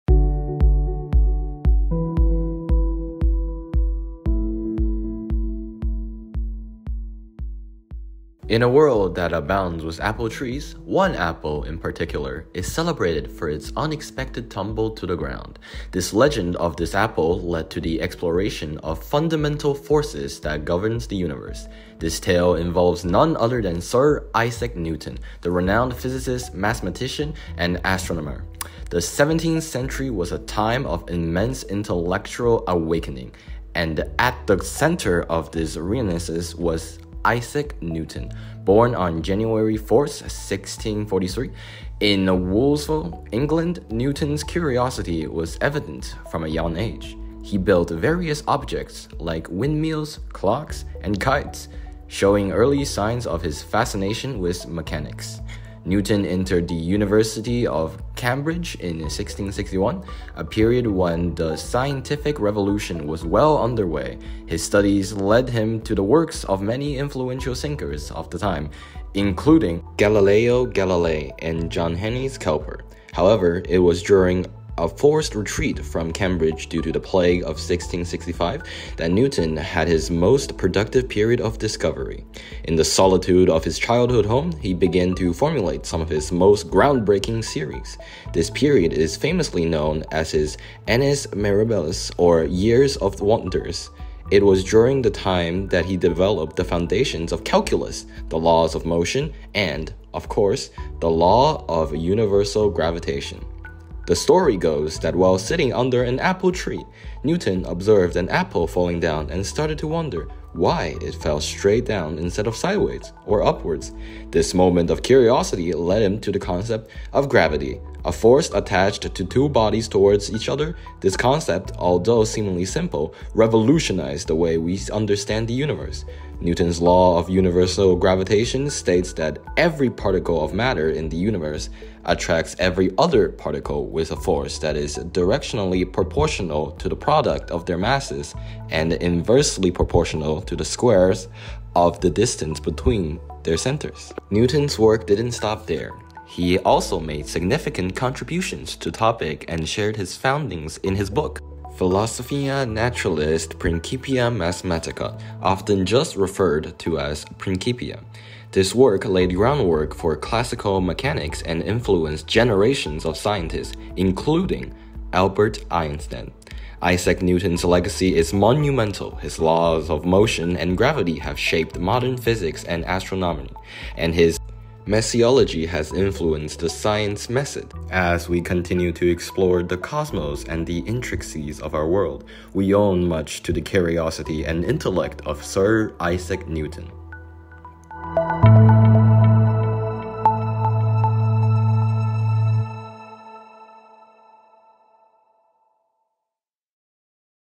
Isaac Newton: The Apple, The Gravity, & The Genius
Dive into the fascinating world of Sir Isaac Newton 🍎 as we unravel the secrets behind his groundbreaking discoveries in animation! Witness the moment the apple drops, and let's leap into the laws that changed the universe 🌌 forever. Get ready for a vibrant journey through history and science like you've never seen before! 🌟📜